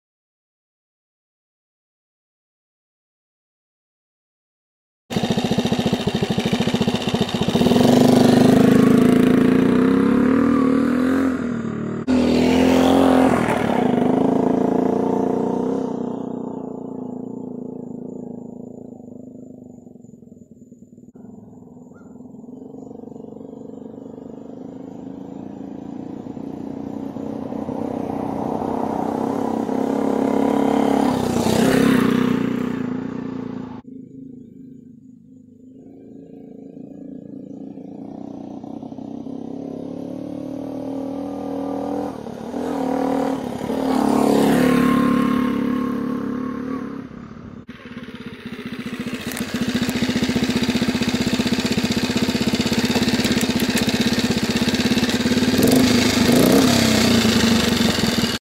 1962 C105T Trail 55 Flyby
This is a nice crusty old project bike I just picked up. Typical Honda after 60 years of neglect a good carb rebuild has it running and driving just. Yes it's a 1962 model, I was able to find the date tag on the wiring harness to verify the year.